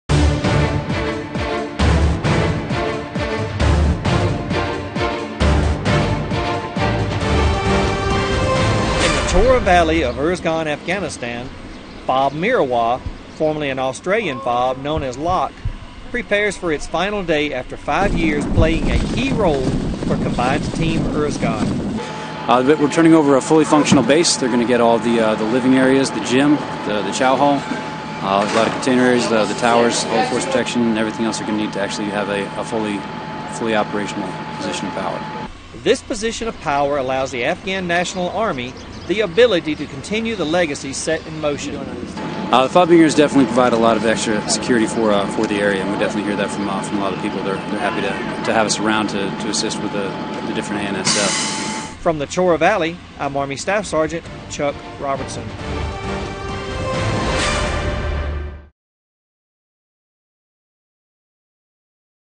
Soldiers Update: FOB Mirwais
Soldiers prepare Foward Operating Base Mirwais for it's closure and turnover to the Afghan National Army.